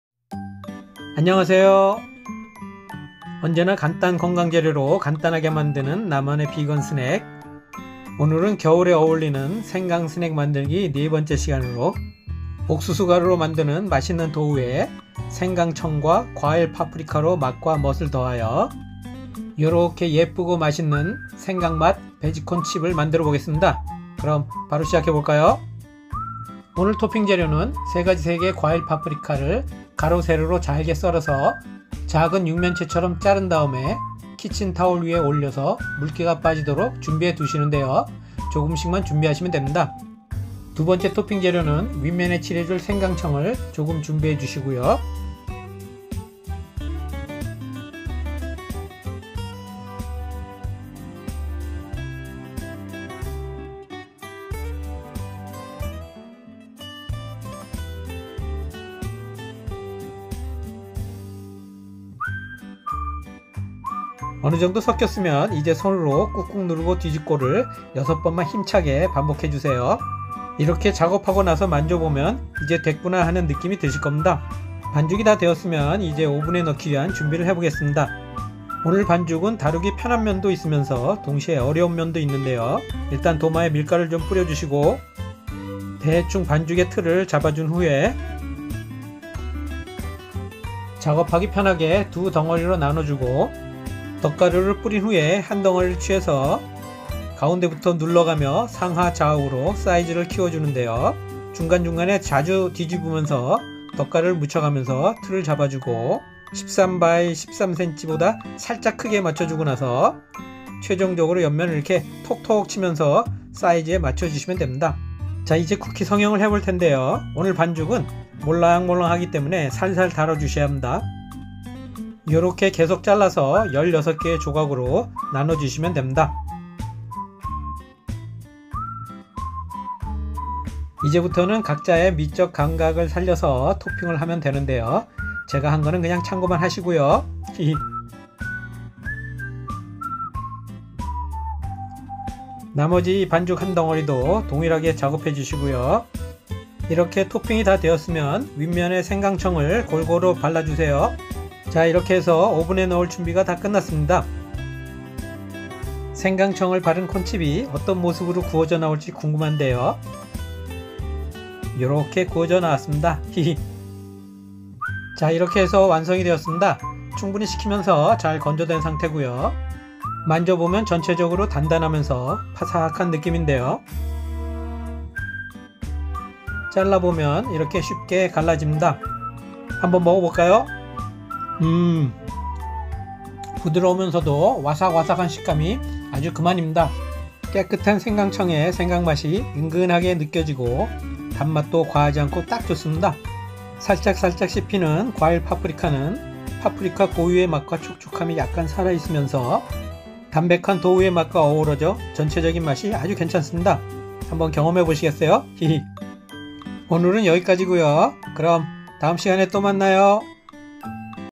(Vegan Snack) 와삭와삭 기분 좋은 생강스낵~ 생강베지콘칩 / 노버터 / 노밀크 / 노에그 /옥수수가루 / 생강청 / 과일파프리카 / 오븐베이킹
안녕하세요~~ 언제나 간단 건강재료로 간단하게 만드는 나만의 비건스낵, 오늘은 겨울에 어울리는 생강스낵 만들기 네번째 시간으로, 옥수수가루로 만드는 맛있는 도우에 생강청과 과일 파프리카로 맛과 멋을 더하여, 요렇게 예쁘고 맛있는 생강베지콘칩을 만들어 보겠습니다. 그럼 바로 시작해 볼까요?

준비 #1 : 토핑재료-1 준비 (과일 파프리카)
3가지 색의 과일 파프리카를 가로 세로로 잘게 썰어서 작은 육면체처럼 자른 다음에, 키친타올 위에 올려 물기가 빠지도록 준비해 두시는데요, 조금씩만 준비하시면 됩니다

준비 #2 : 윗면 도포용 생강청 (약 2T 정도)
준비 #3 : 가루재료 준비 (옥수수가루 90g, 박력분 45g, 베이킹파우더 6g을 섞어주세요)
준비 #4 : 액체재료 준비 (두유 65g, 카놀라유 2T, 설탕 30g, 소금 1g을 섞어주세요)
준비 #5 : 가루재료를 액체재료에 넣고 섞어주세요

(작업순서)
- 반죽이 다 되었으면 이제 오븐에 넣기 위한 준비를 해보겠습니다
- 오늘 반죽은 다루기 편한 점도 있으면서 동시에 어려운 면도 있는데요
- 우선 도마에 밀가루를 뿌려주고
- 대충 반죽의 틀을 잡아준 후에
- 작업하기 편하게 두 덩어리로 나눠주고
- 덧가루를 뿌린 후에 한 덩어리를 취해서
- 가운데부터 눌러가며 상하좌우로 사이즈를 키워주는데요
- 중간중간에 자주 뒤집으면서 덧가루를 묻혀가면서 틀을 잡아주고
- 13cm x 13cm 보다 살짝 크게 맞춰주고 나서
- 최종적으로 옆면을 이렇게 톡톡~ 치면서 사이즈에 맞춰주시면 됩니다
- 자 이제 쿠키성형을 해볼텐데요, 오늘 반죽은 몰랑몰랑하기 때문에 살살 다뤄주셔야 합니다
- 이제부터는 각자의 미적 감각을 살려서 토핑을 하면 되는데요, 제가 한거는 그냥 참고만 ㅎㅎ
- 나머지 반죽 한 덩어리도 동일하게 작업해 주시구요
- 이렇게 토핑이 다 되었으면 윗면에 생강청을 골고루 발라주세요
- 180도로 예열된 오븐모드로 25분^^

* 옥수수가루 : 초야식품
* 생강청 : 백설 리얼생강청
* 두유 : 베이밀 에이
* 설탕 : 백설 흡수를 줄인 자일로스 설탕 (화이트)

자~ 이렇게 해서 완성이 되었습니다. 충분히 식히면서 잘 건조된 상태구요~ 만져보면 전체적 으로 단단하면서 파삭한 느낌인데요, 잘라보면 이렇게 쉽게 갈라집니다. 한번 먹어 볼까요? 음~~ 부드러우면서도 와삭와삭한 식감이 아주 그만입니다. 깨끗한 생강청의 생강맛이 은근하게 느껴지고, 단맛도 과하지 않고 딱 좋습니다. 살짝 살짝 씹히는 과일 파프리카는 파프리카 고유의 맛과 촉촉함이 약간 살아 있으면서~ 담백한 도우의 맛과 어우러져 전체적인 맛이 아주 괜찮습니다. 한번 경험해 보시겠어요? ㅎㅎ 자세한 조리 방법은 영상속의 설명과 자막을 참고해 주시구요~ 그럼 다음 시간에 또 만나요~